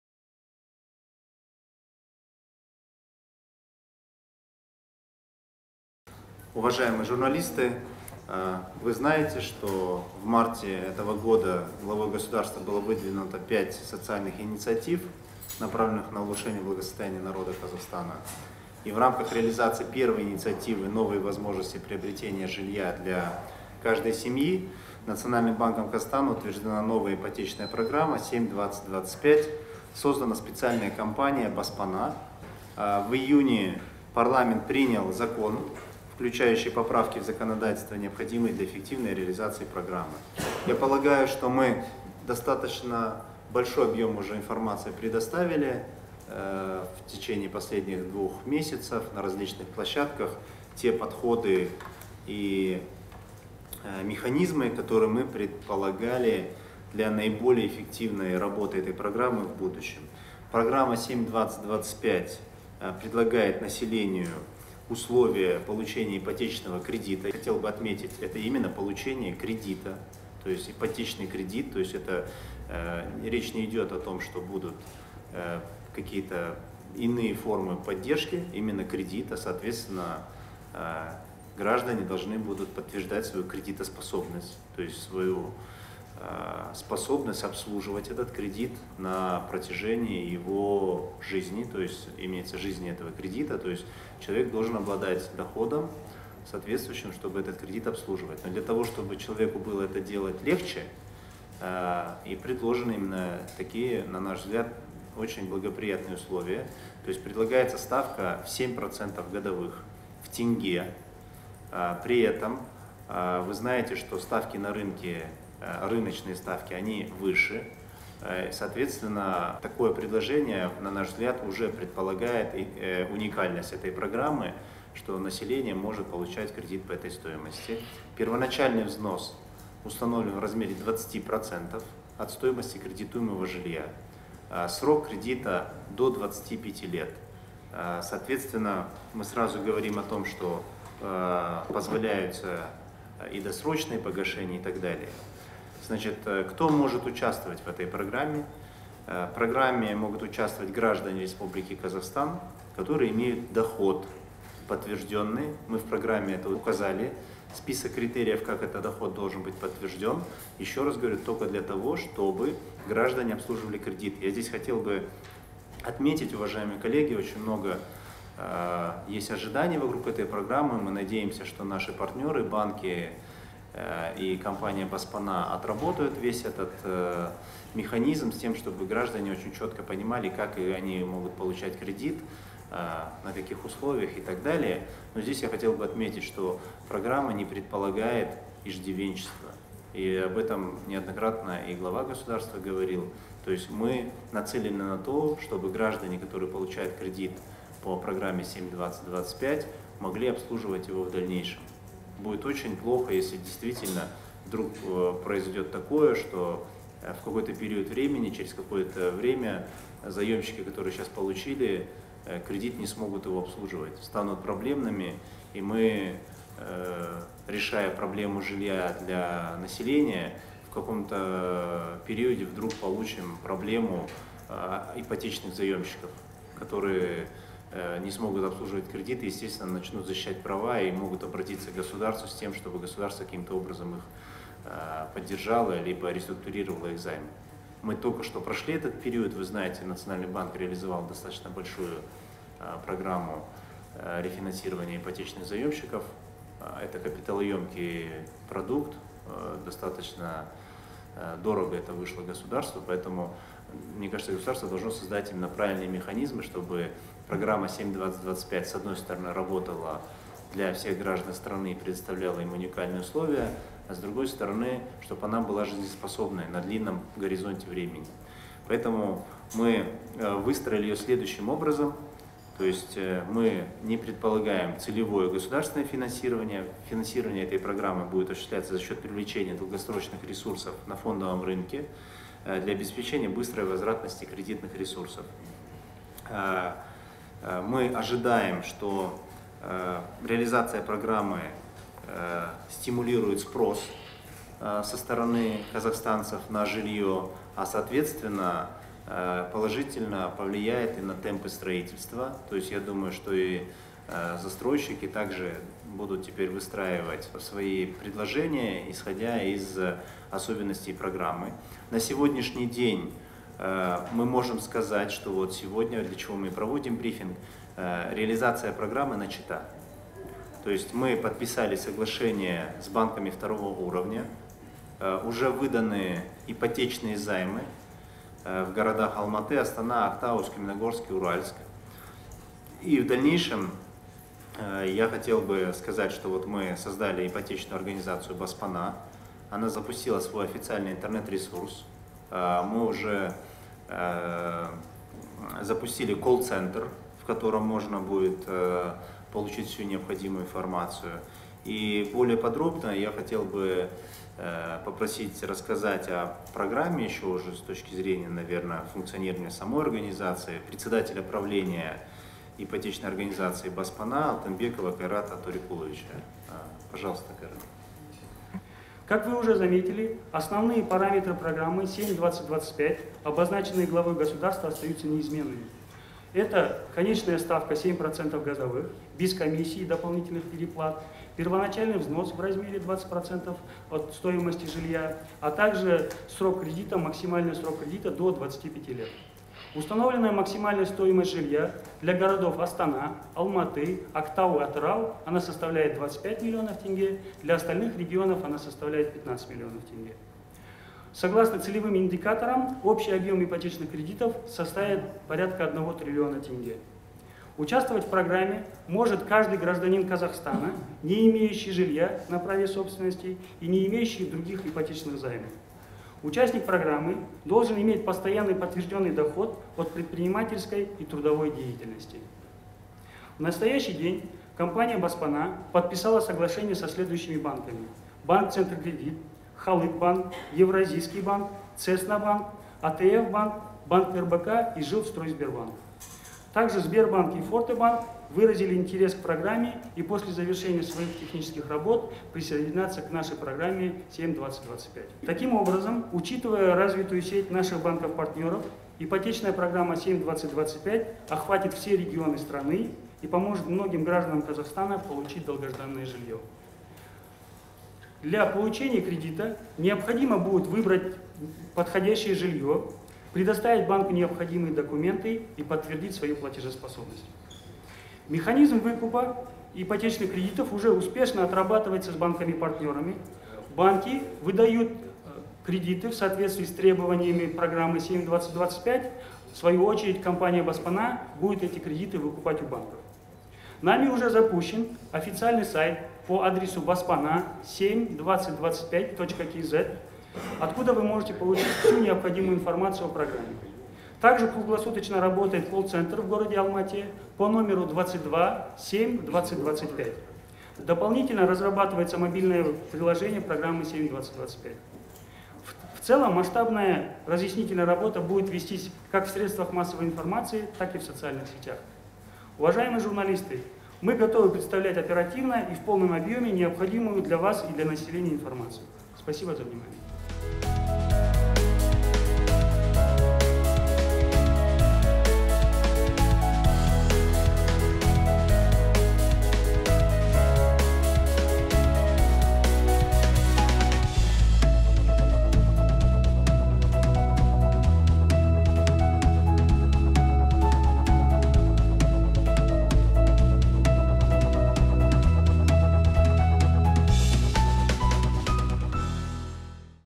Видео с брифинга Национального Банка Казахстана и ипотечной организации «Баспана»
Представляем Вашему вниманию видео с брифинга Национального Банка Казахстана и ипотечной организации «Баспана», который состоялся 4 июля 2018 года в г. Алматы.